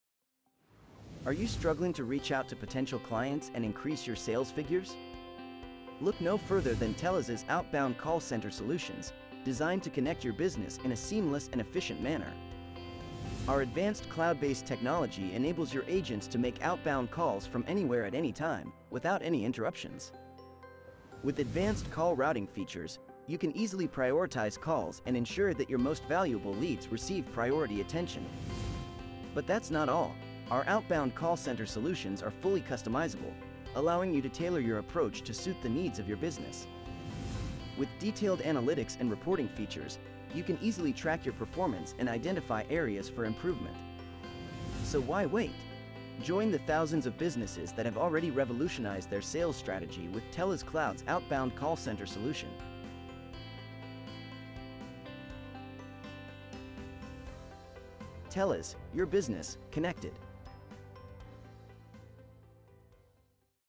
"Outbound Call Center Mastery: Elevate Customer Outreach"
Welcome to Teloz informative video that delves into the definition of Outbound Call Centers. As a leader in communication solutions, Teloz is excited to guide you through the world of Outbound Call Centers, where proactive communication takes center stage.